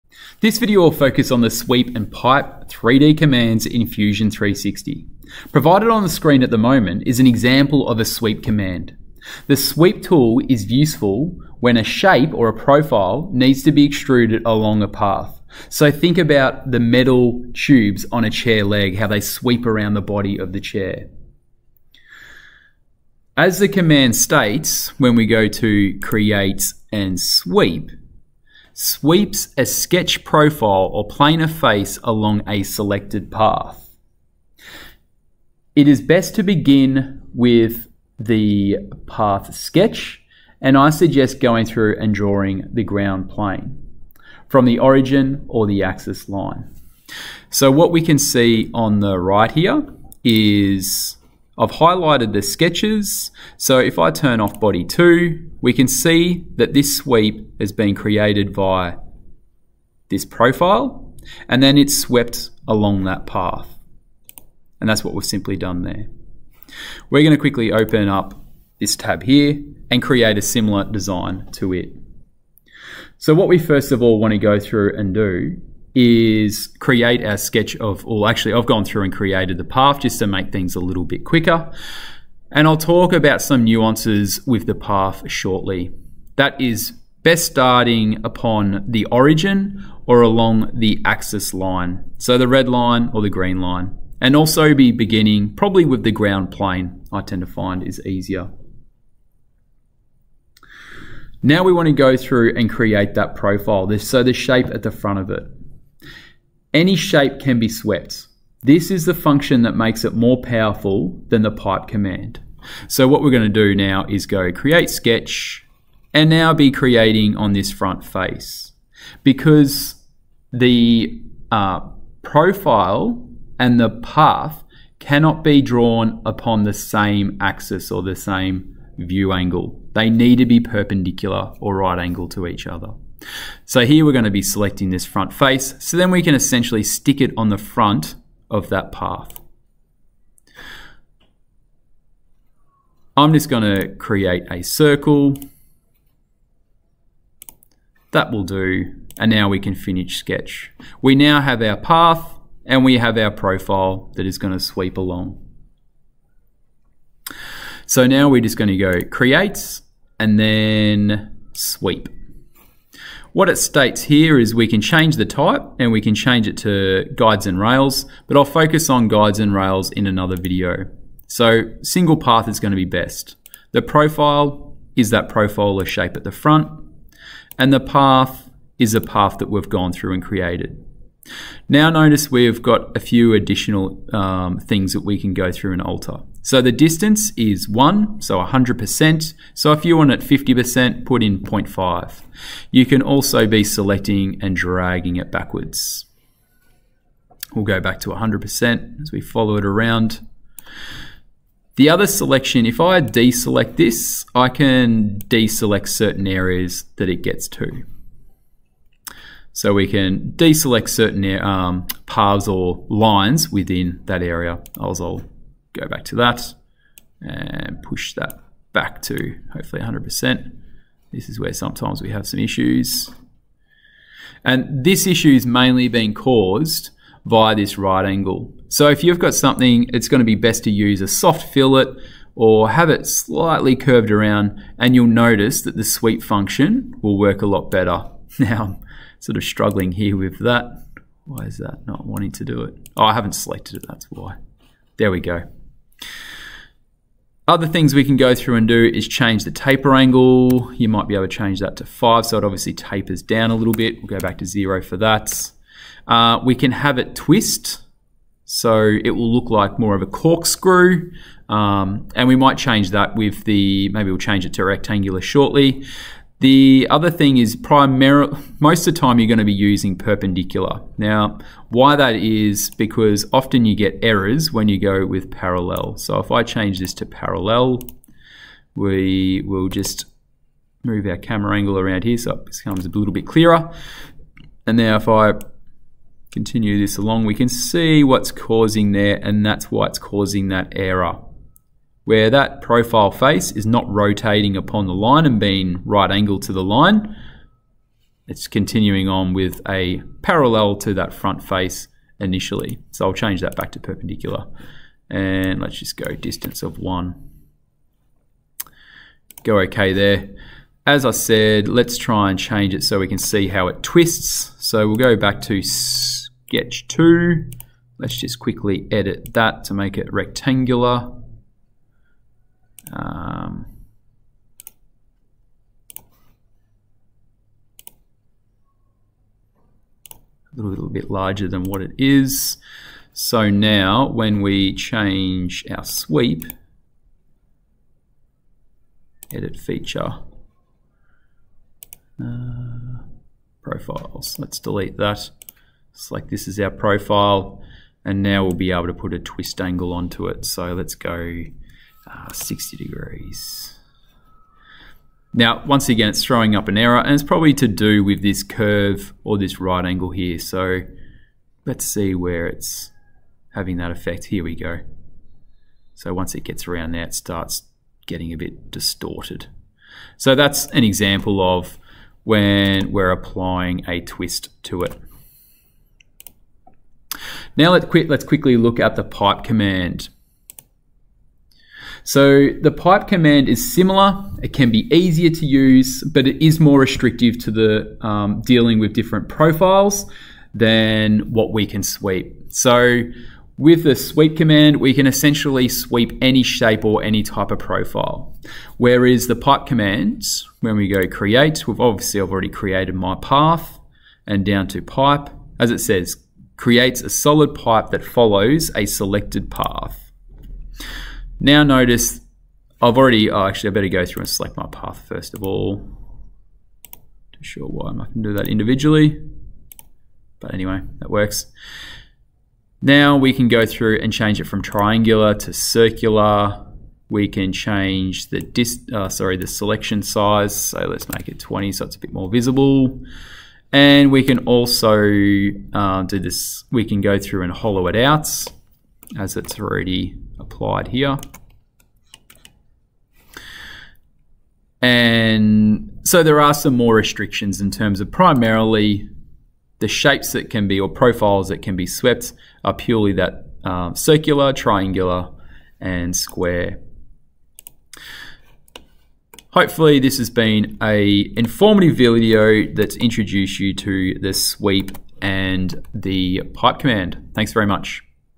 Sweep vs Pipe - Fusion 360
Introduction 0:00
Sweep needs - Path and profile 0:24
Path Sketch 0:37
New sweep demonstration 1:07
Path Suggestions 1:30
Drawing Profile / Shape 1:45
Sweep Command 2:39
Taper Angle 4:08
Orientation parallel vs perpendicular 4:30
Twist Angle 5:40
Pipe 6:22
Hollow pipe 7:15